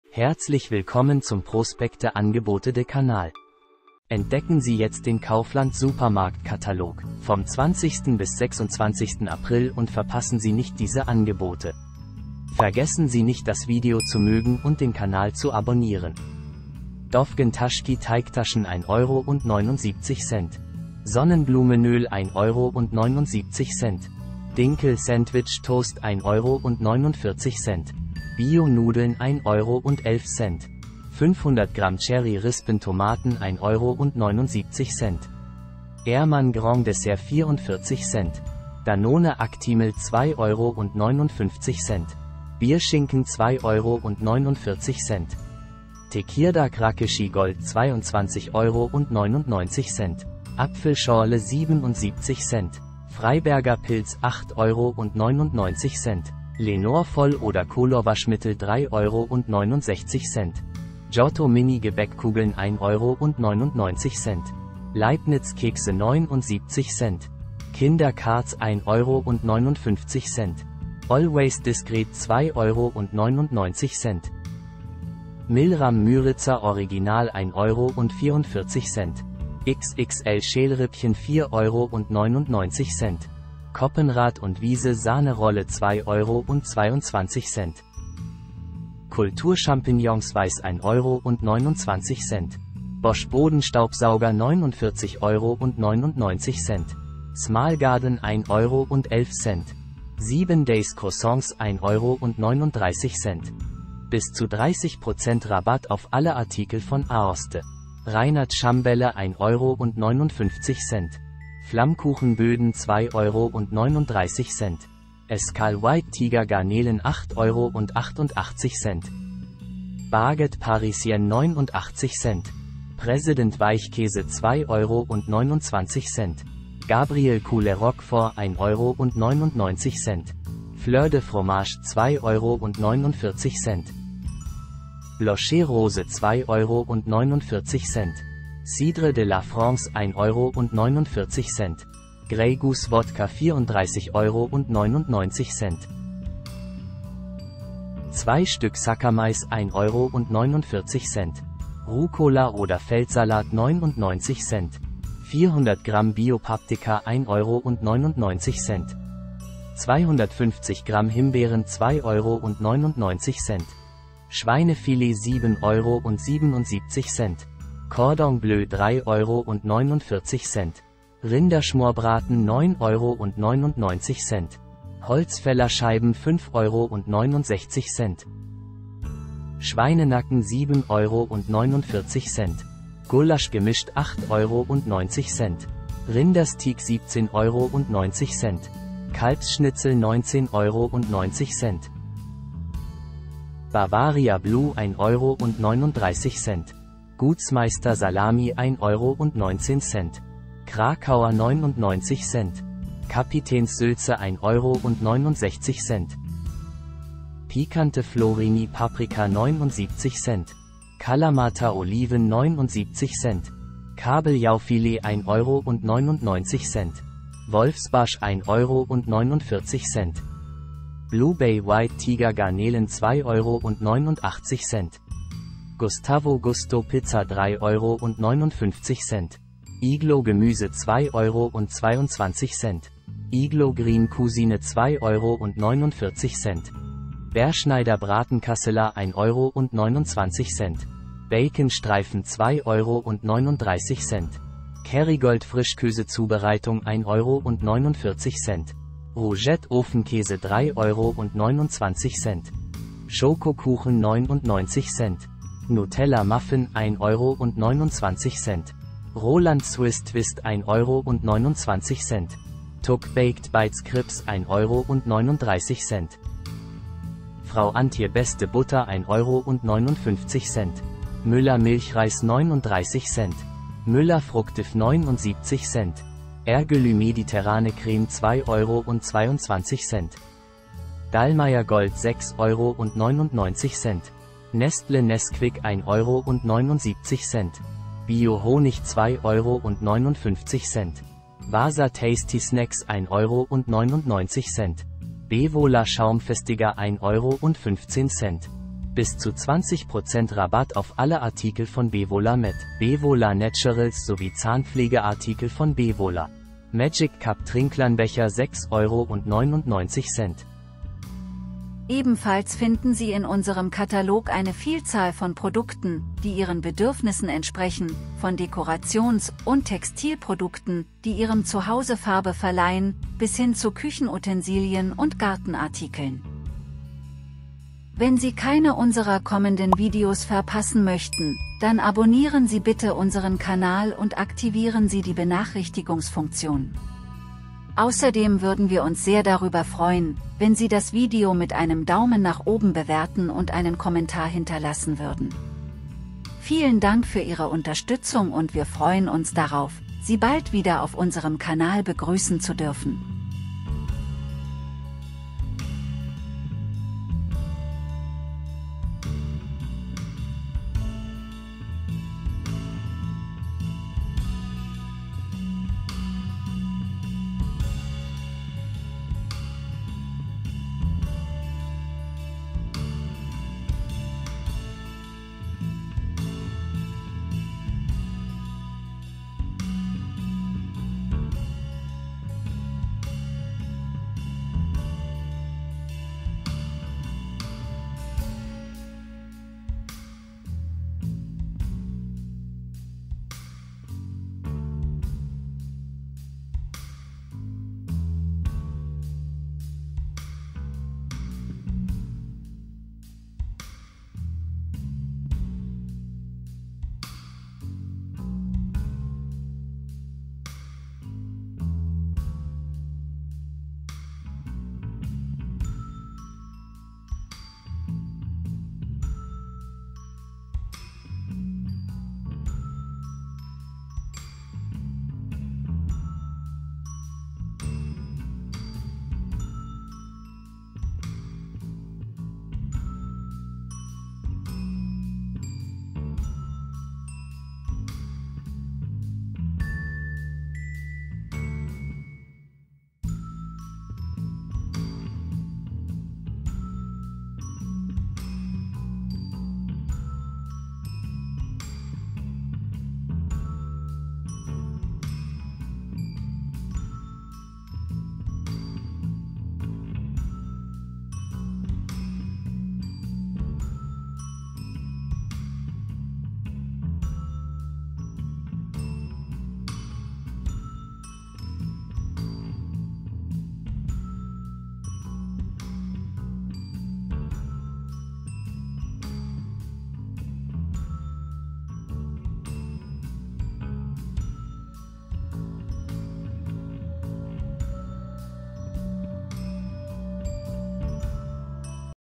KAUFLAND Prospekt | Angebote und Aktionen | Gültig von 20.04.2023
Der neue Kaufland Prospekt der Woche (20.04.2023. – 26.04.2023) ist da! Jetzt alle Angebote aus den Prospekten von Kaufland online entdecken.